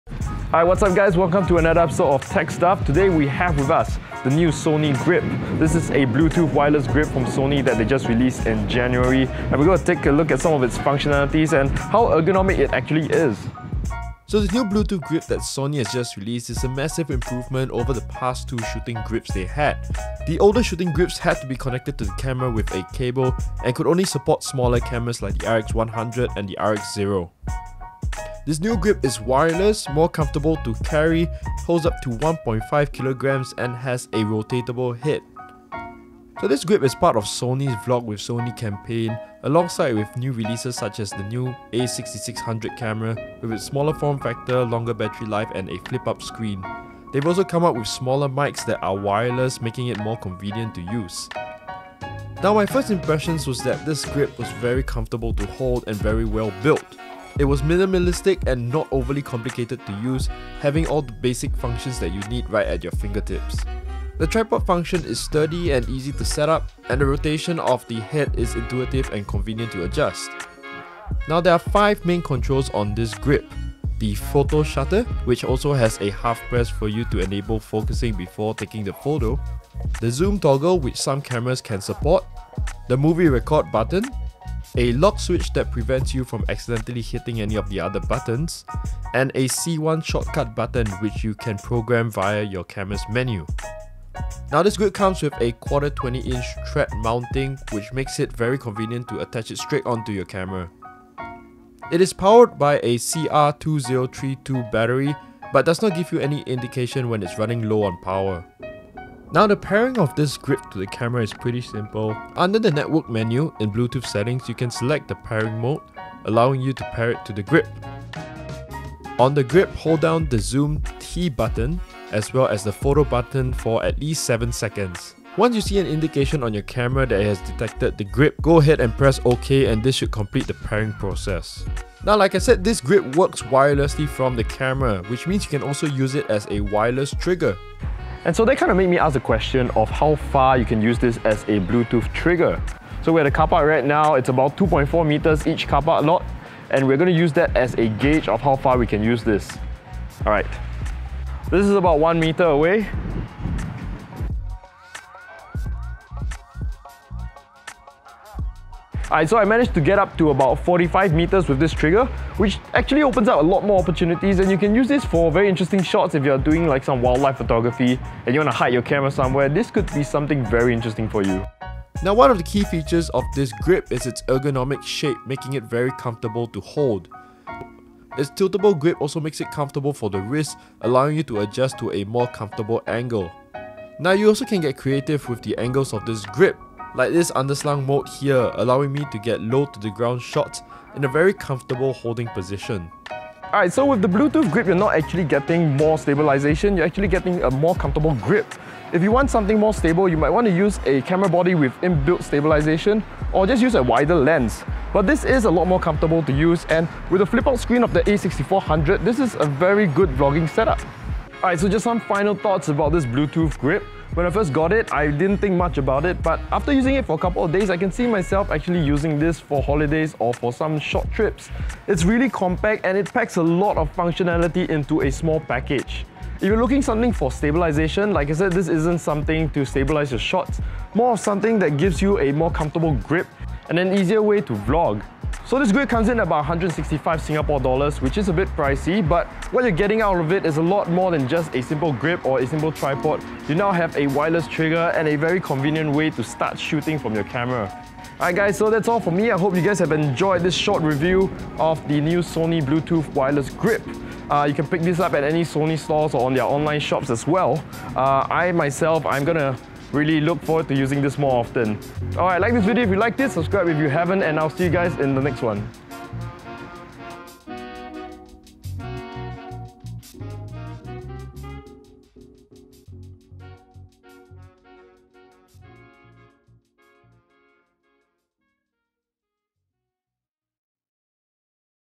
The Future of Vlogging | Sony Bluetooth Wireless Grip GP-VPT2BT
In this episode we take a look at Sony's latest shooting grip that is wireless and does so much more than just provide a comfortable grip. The shooting grip comes with 5 key functions that can also be used detached from the camera like a remote trigger. Is this the future of vlogging?